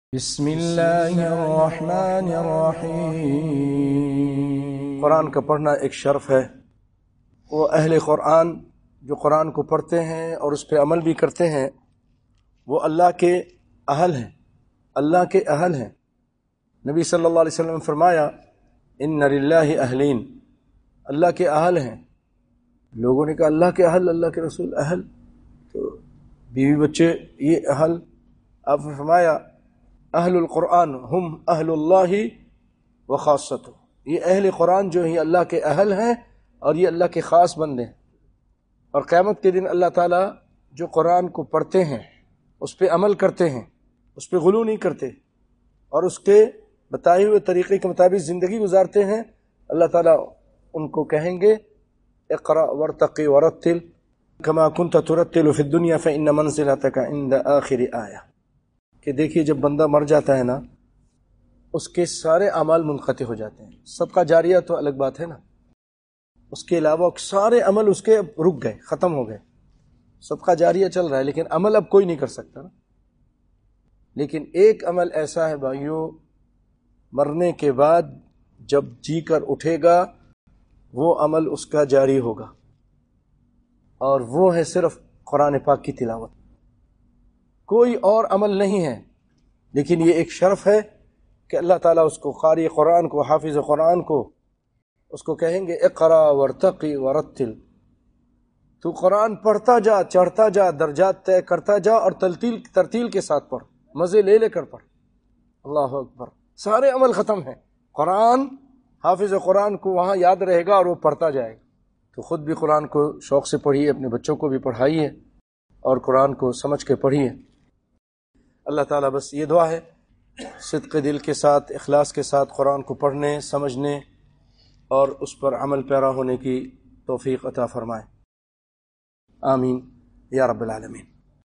Mout kay baad jari rehnay wala amal - Sh Maaz Ahmed
Aik aaisa amal, jo mout aanay kay baad bhi insan karayga
Shaikh Maaz Ahmed Farooqi ki zabani